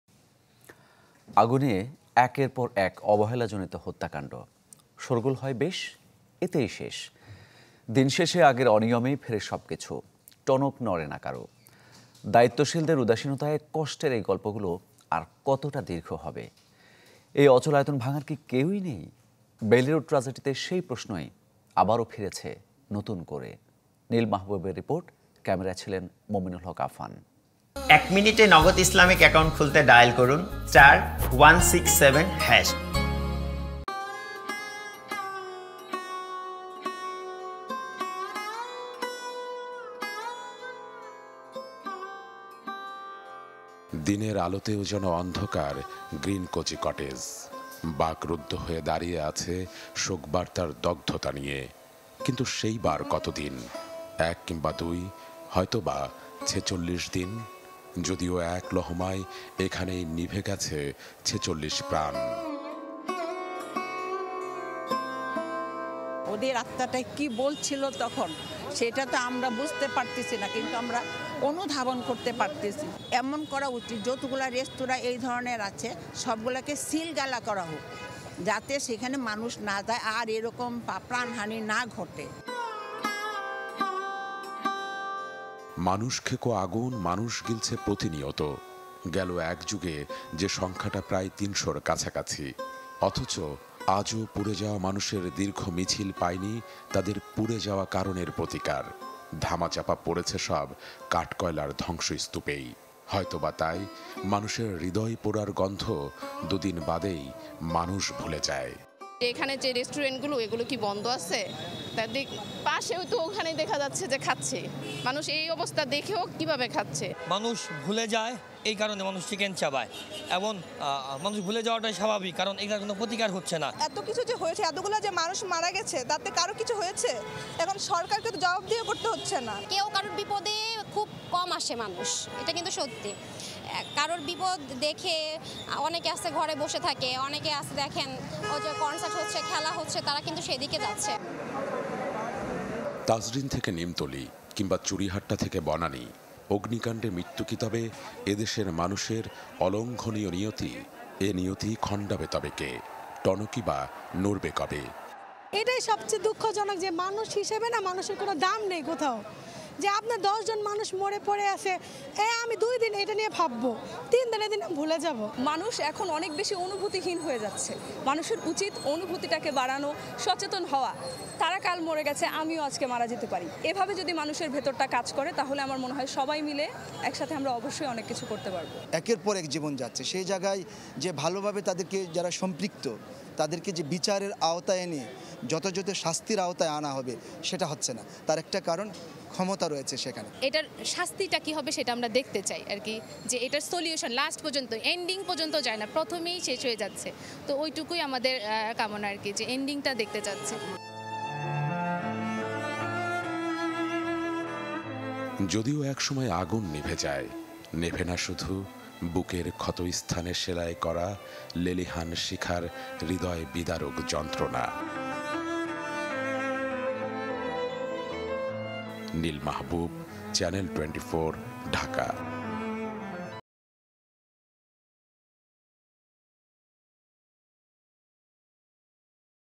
অগ্নিকাণ্ডে মৃত্যু কী তবে, এদেশের মানুষের অলঙ্ঘনীয় নিয়তি? | Bailey Road | Fire Incident | Channel 24
আগুনে একের পর এক অবহেলাজনিত হত্যাকাণ্ড। শোরগোল হয় বেশ, এতেই শেষ। দিনশেষে আগের অনিয়মেই ফেরে সবকিছু। টনক নড়ে না কারো। দায়িত্বশীলদের উদাসীনতায় কষ্টের এই গল্পগুলো আর কতটা দীর্ঘ হবে? এ অচলায়তন ভাঙার কি কেউ নেই? বেইলি রোড ট্র্যাজেডিতে সেই প্রশ্নই আবার ফিরেছে, নতুন কোরে। নীল মাহাবুবের রিপোর্ট। ক্যামেরায় ছিলেন, মুমিনুল হক আফান।

»» Office Address:
Bangladesh.